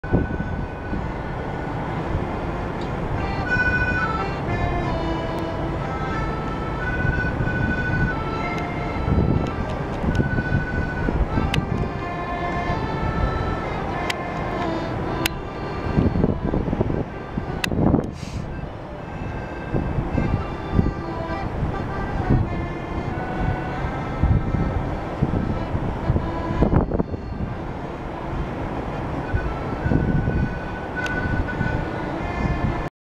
Old dude playing accordion in the park in Burnaby
Working from home, when I hear accordion music. Look outside (15 stories down), and there's some guy practicing away